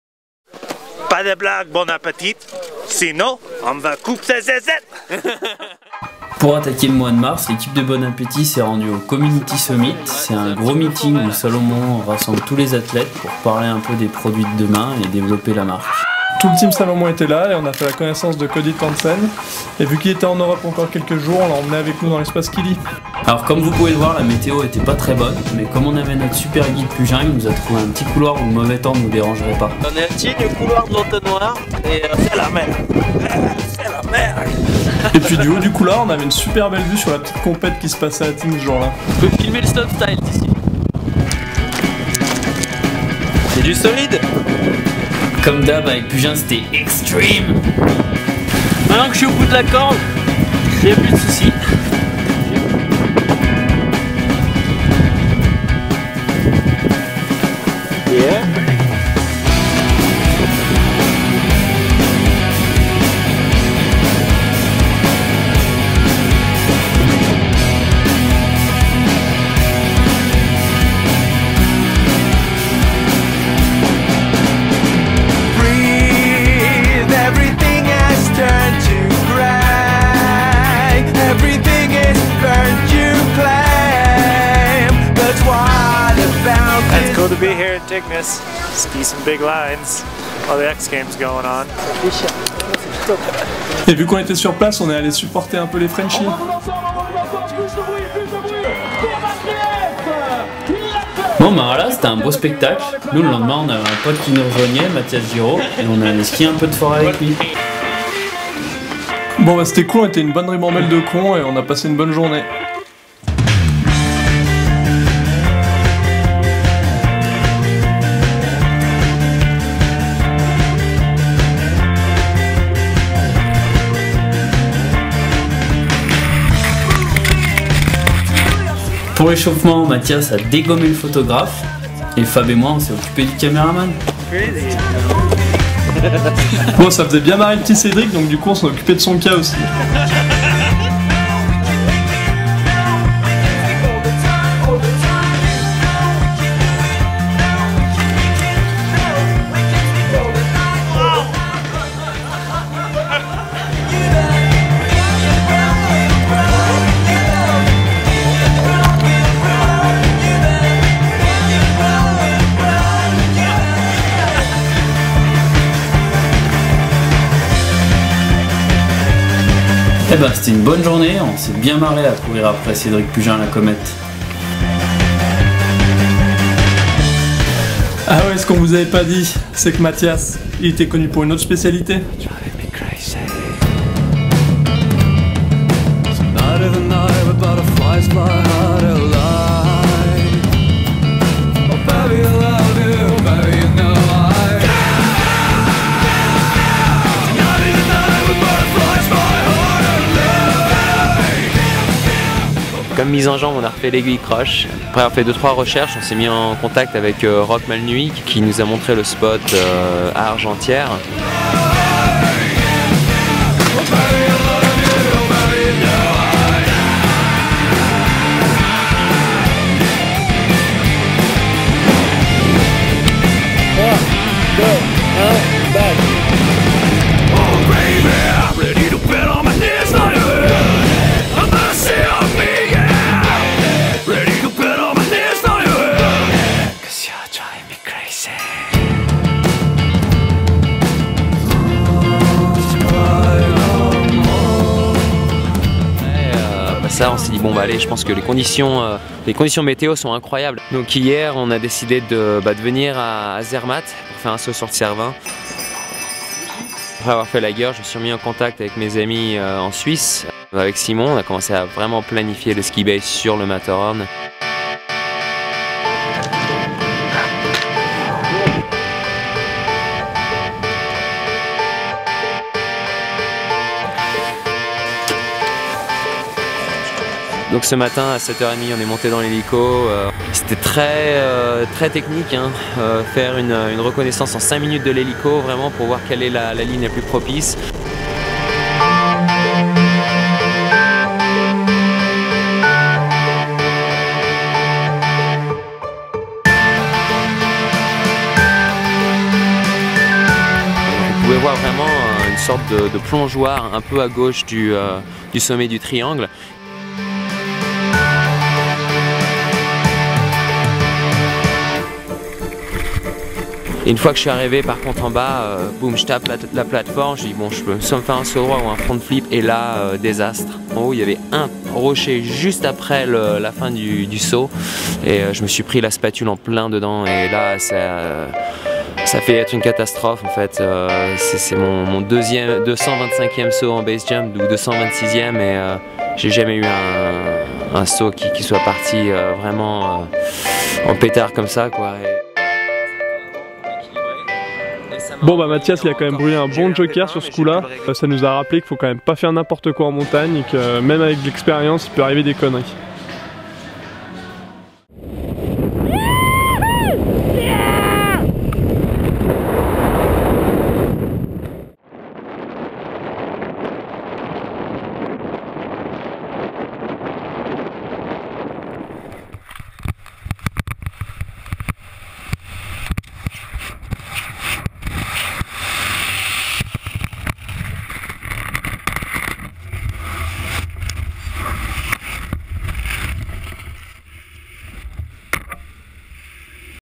Episode 6 - Bon Appétit - Ski Base !
Pour ce nouvel épisode nous avons glissé nos lattes sur les couloirs surplombants Tignes et ses fameux X Games. Parfait pour apprécier le spectacle, toucher un peu de bonne neige et encourager les frenchies là bas.

La suite des événements, c'est une tout autre vision de notre sport que nous présente Mathias Giraud, on vous laisse déguster tout ça dans l'épisode..